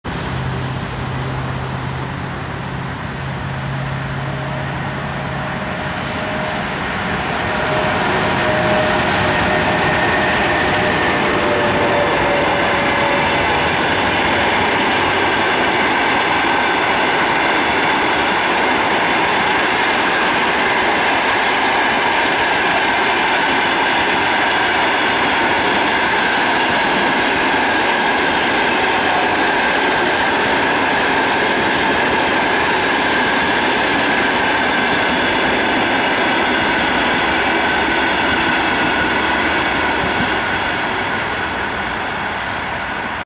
Электровоз ЭП1М-538 с поездом №382 Москва — Грозный
Электровоз ЭП1М-538 (приписан ТЧЭ-3 Россошь Ю-ВОСТ) с пассажирским поездом №382 Москва — Грозный с перегона Ростов-Товарный - Зоологический Сад заходит на разъезд Зоологический Сад, Лиховское направление пути, г. Ростов-на-Дону, Северо-Кавказская железная дорога, Россия, 14:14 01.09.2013 г.